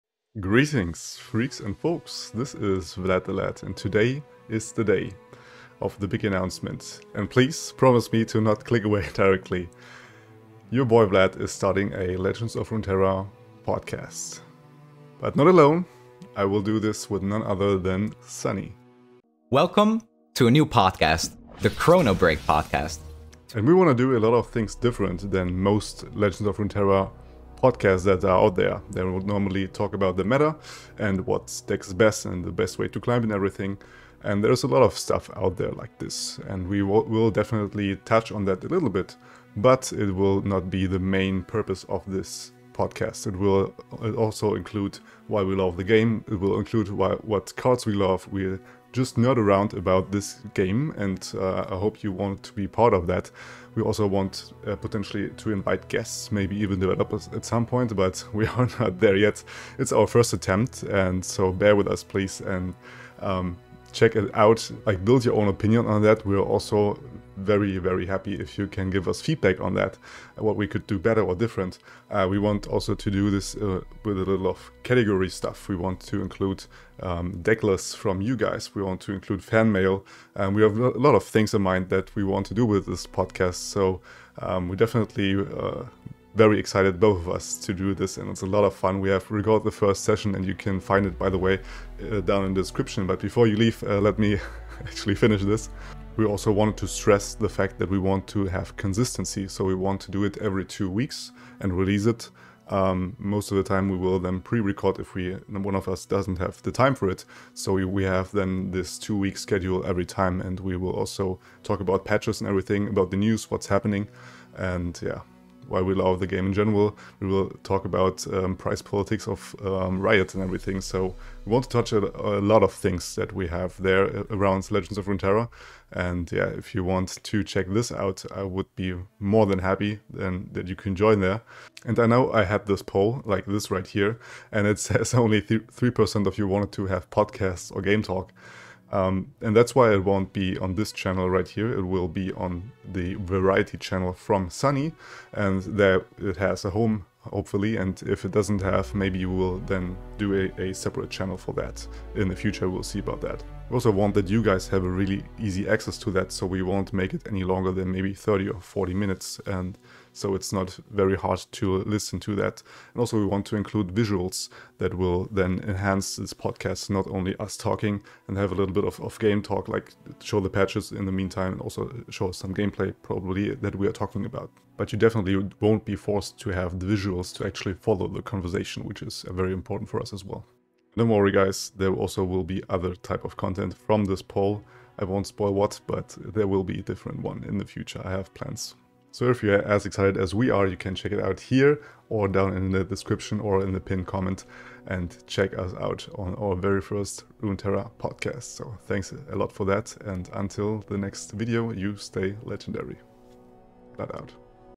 BIG Announcement
This project has been in the making for quite some time. We´re happy to announce the new Legends of Runeterra Podcast "The Chronobreak Podcast".